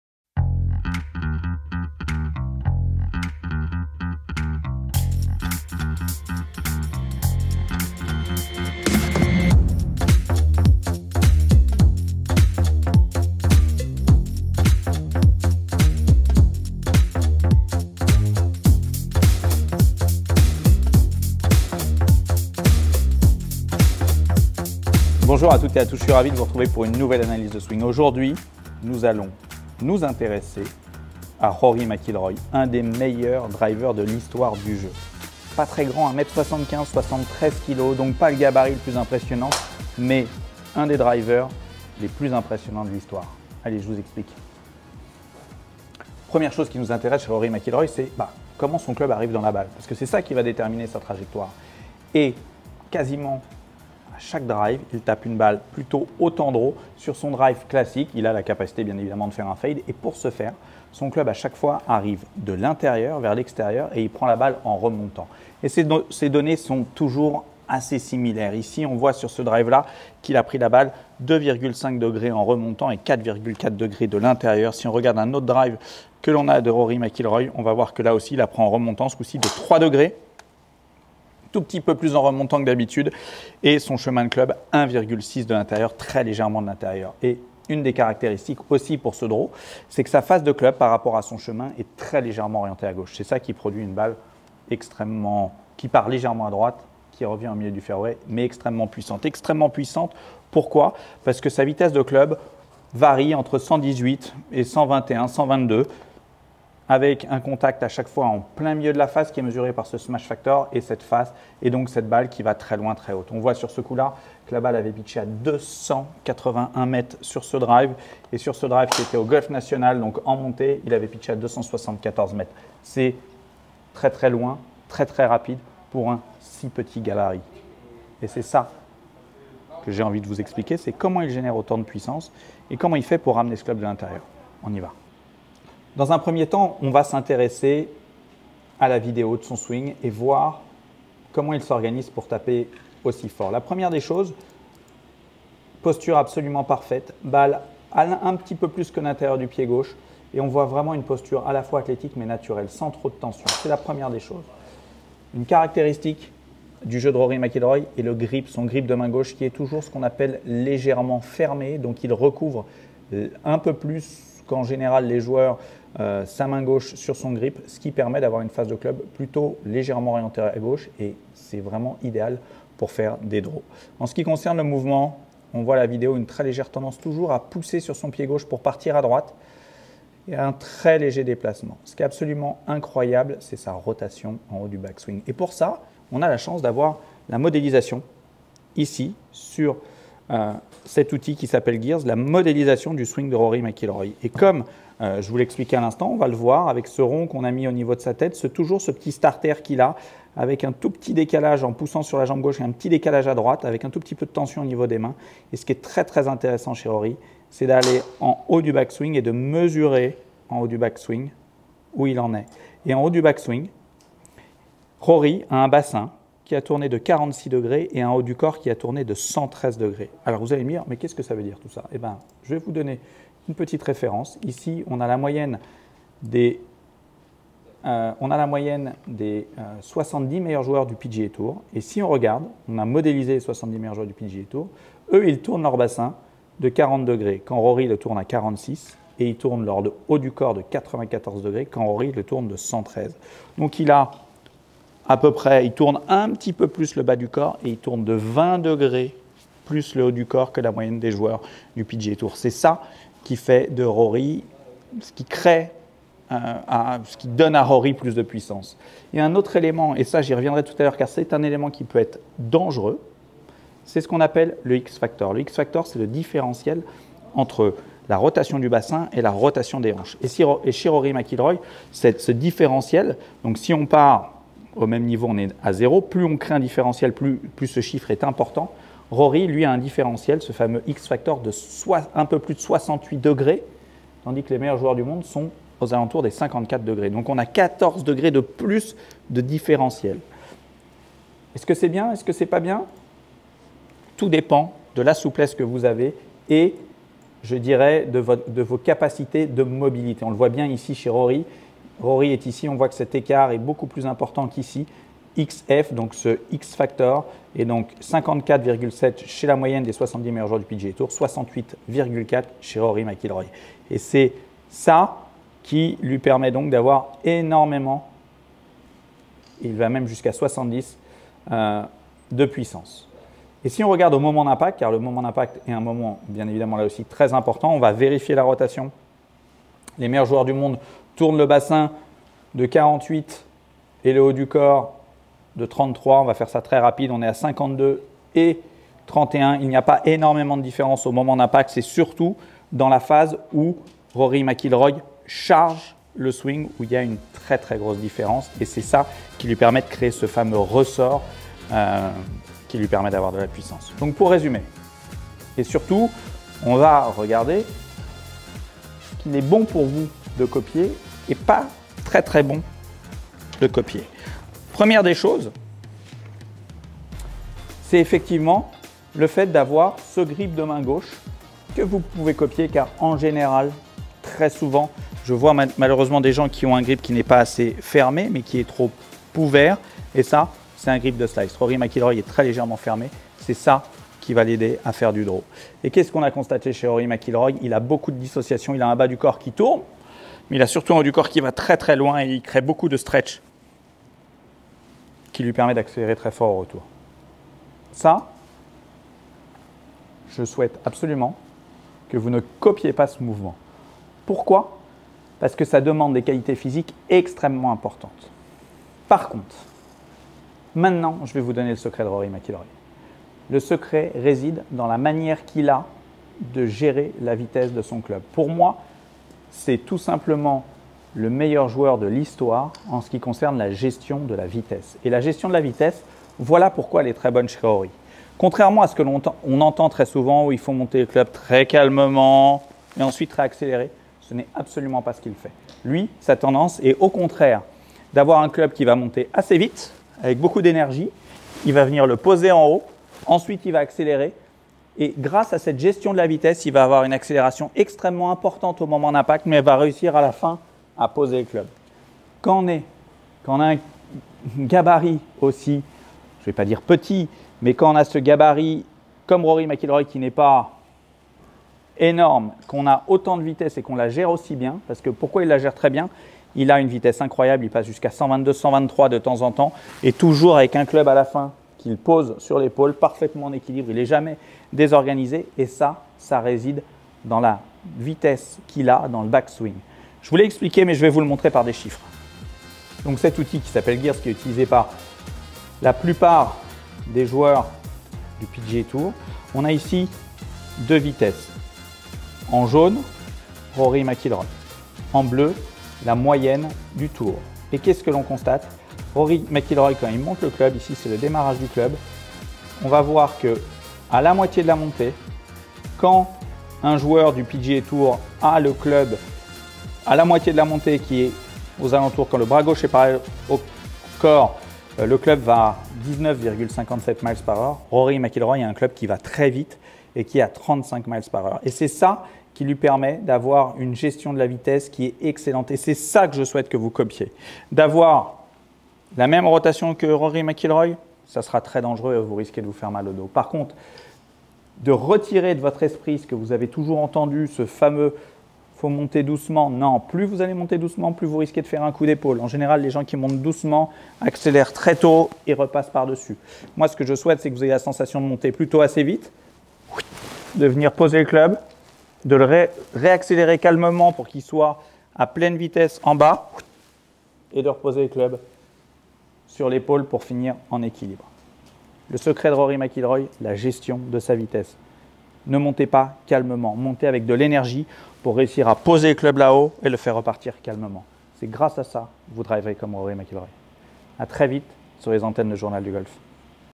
COMMENT RORY MCILROY FAIT-IL POUR TAPER AUSSI FORT ??
Rory McIlroy a terminé la saison 2022 dans la peau de numéro un mondial. Un trône planétaire que le Nord Irlandais a re conquis en s'appuyant encore et toujours sur une frappe de balle hors norme. On vous décrypte son swing.